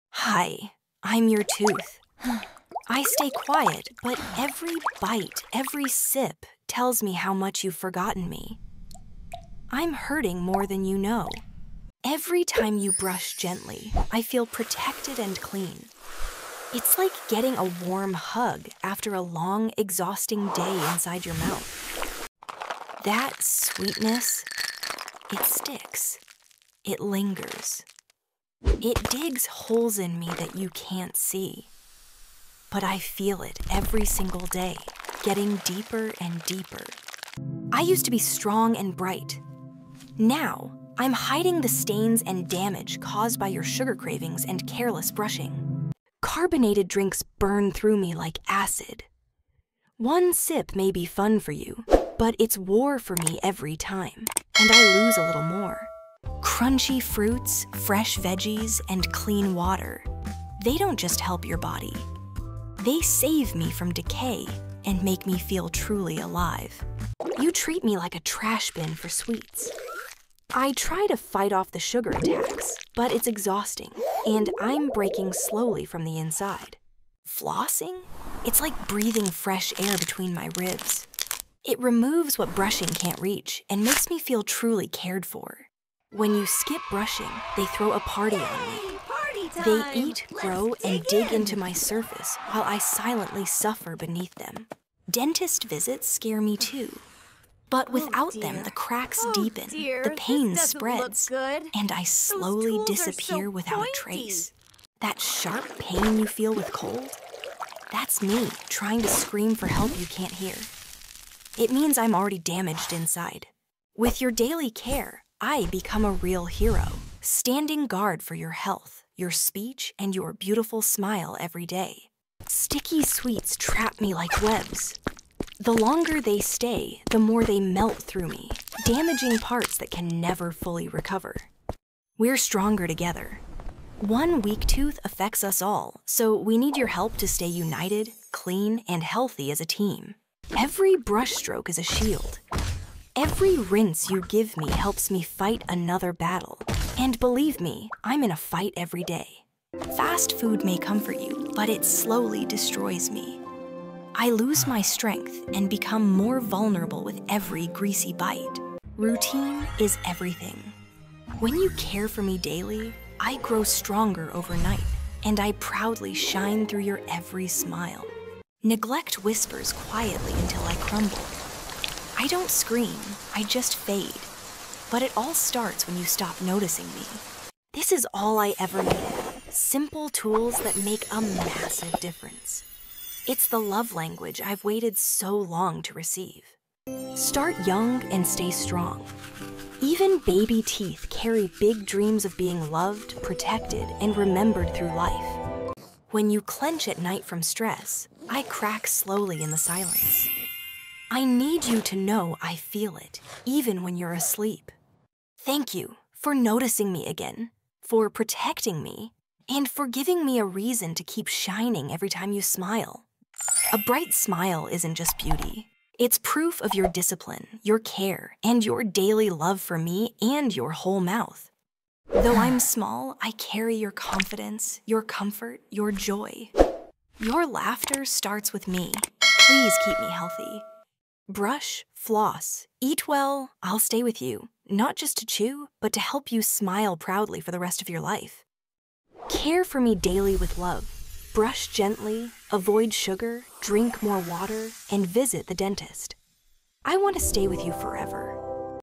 I’m Your Tooth… And I’m Slowly Breaking Inside | Tooth Pain Warning 😢
This emotional animation gives voice to your teeth—sharing what they go through every day with sugar, soda, and neglect.
See the truth from the inside, and discover how daily habits affect your smile.
Protect your oral health before it’s too late.

🎨 Watch till the end for helpful tips to save your teeth!